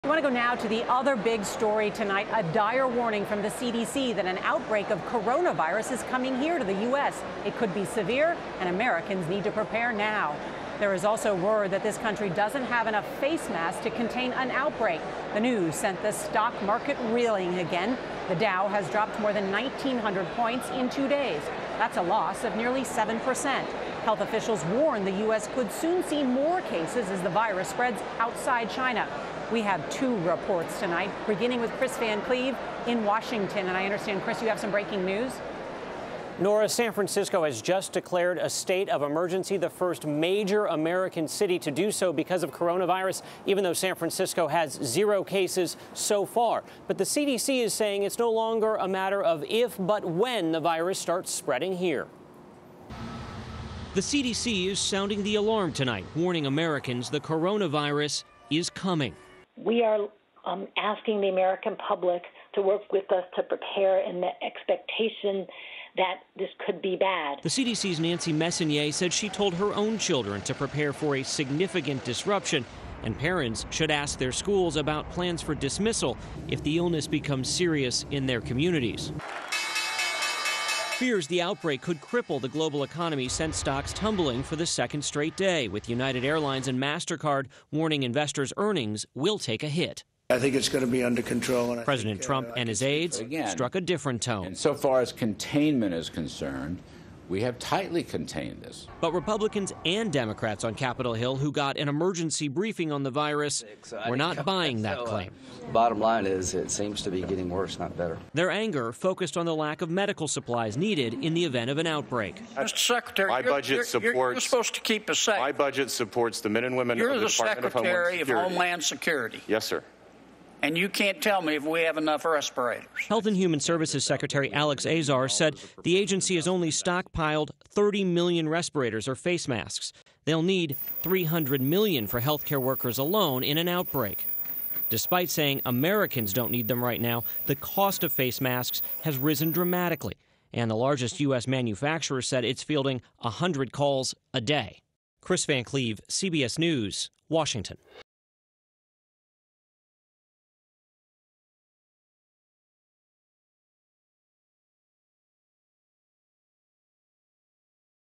U.S. economy takes hit in wake of coronavirus spread
The CDC has issued a warning that the U.S. could see more cases as the coronavirus spreads outside of China. Kris Van Cleave reports.